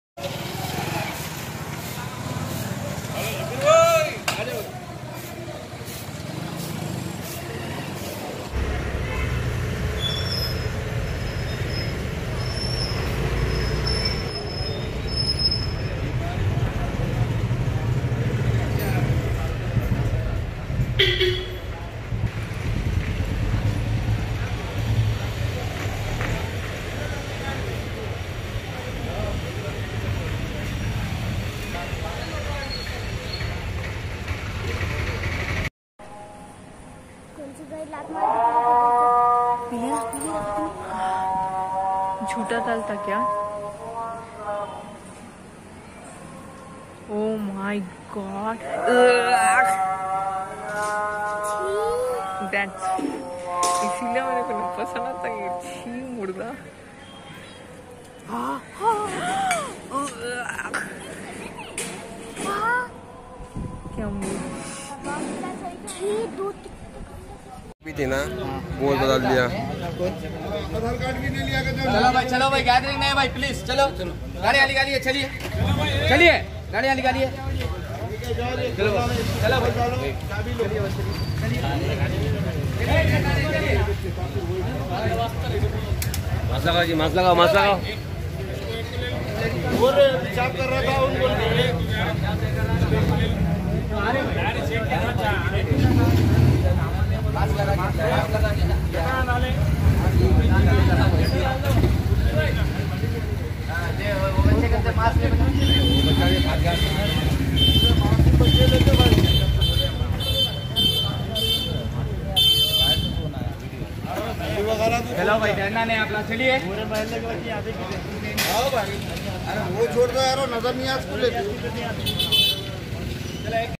F.I.R Booked Against JAHANGEER DAIRY PRODUCTS Darulshifa Golkhabar Branch Cr.No.154/2020 u/s 269, 27
F.I.R Booked Against JAHANGEER DAIRY PRODUCTS Darulshifa Golkhabar Branch Cr.No.154/2020 u/s 269, 272, 273 IPC r/w 34. On The Complaint By Syed Azmath Hussain Jaffery President Public Grievance Association (Regd)*